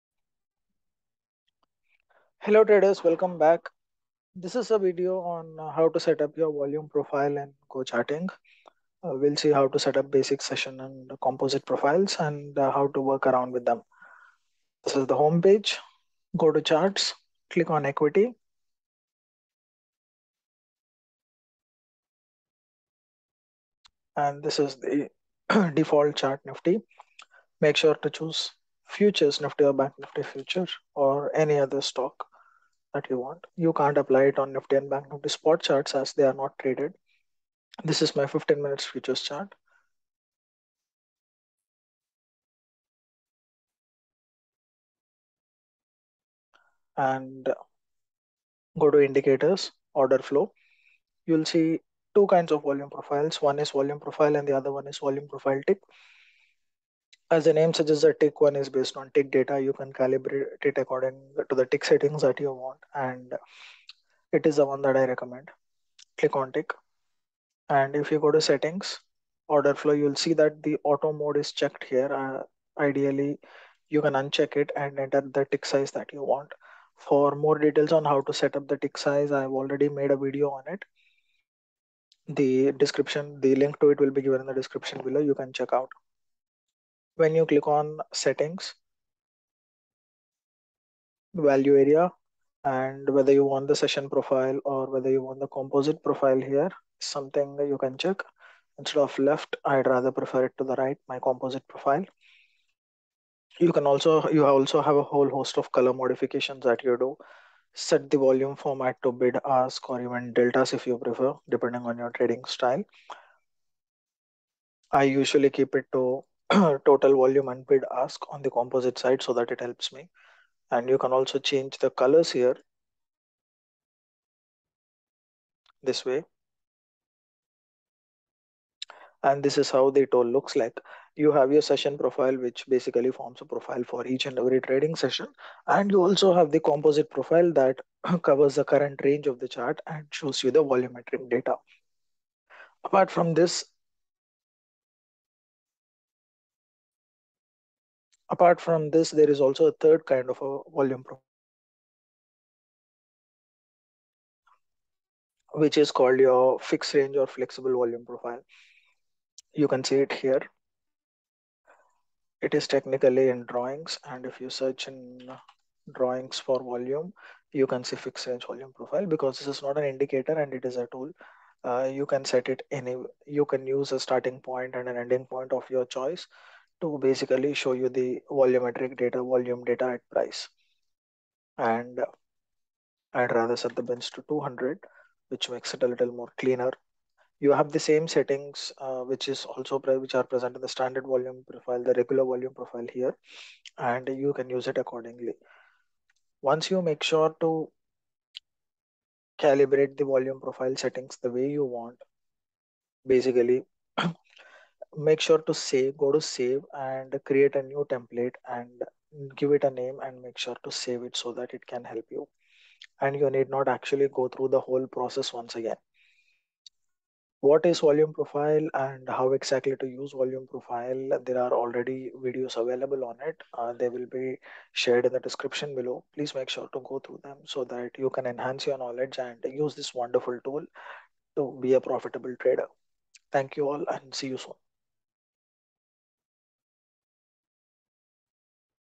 How to set up volume profile charts on GoCharting.com
How to set up volume profile charts?
What is volume profile?
How to use volume profile?
What is composite profile?